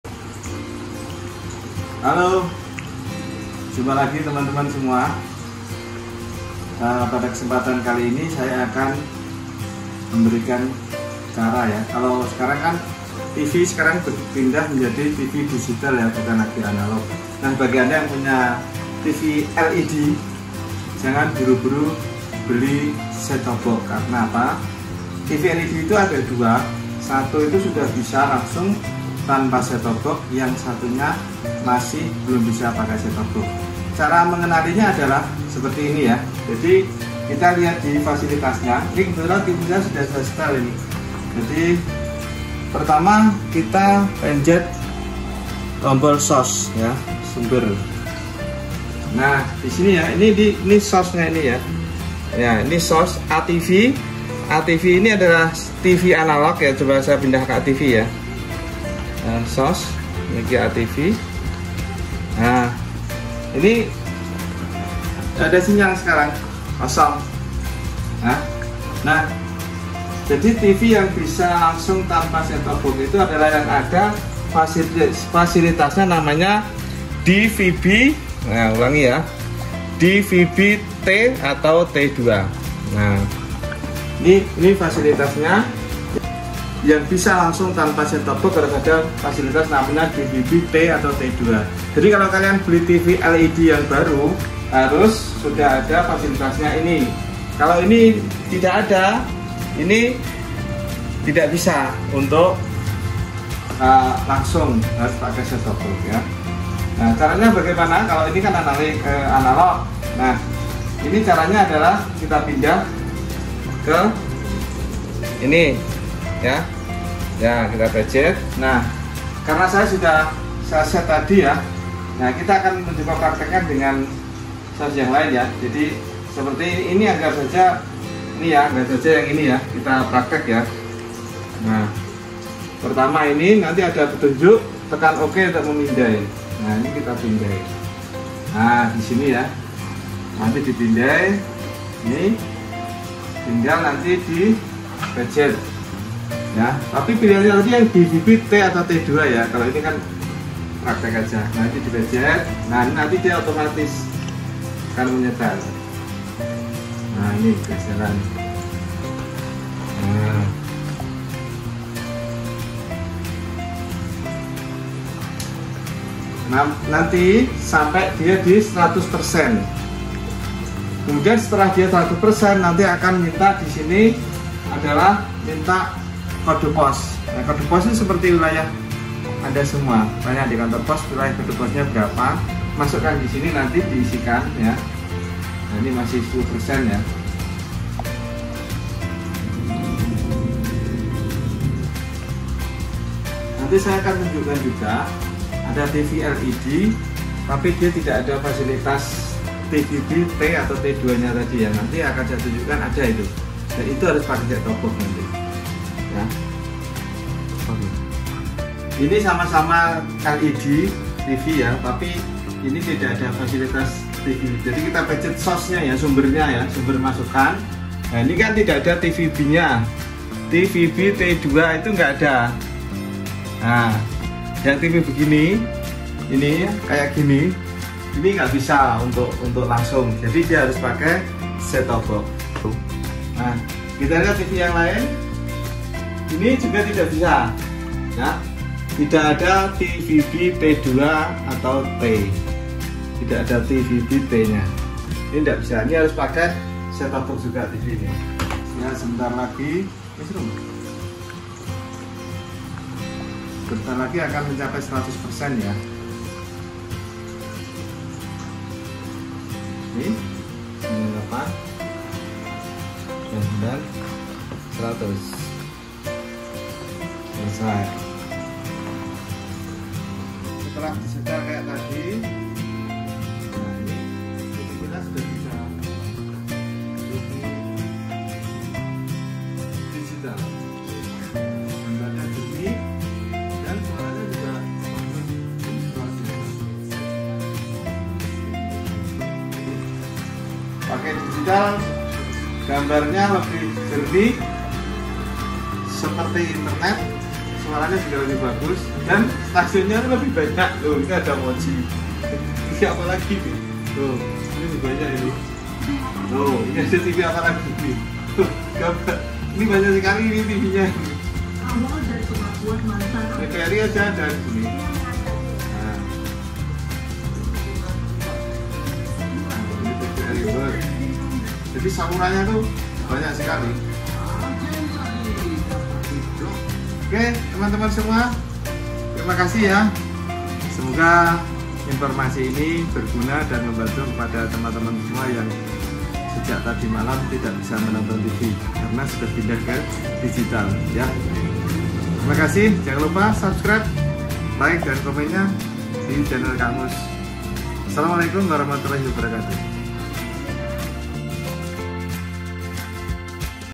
JANGAN BURU2 BELI SET TOP BOX SEBELUM CEK TV LED MU || INI CIRI2 TV LED DIGITAL DAN CARA SETTINGNYA
Video kali ini berisi tentang cara setting tv LED menjadi digital tanpa set top box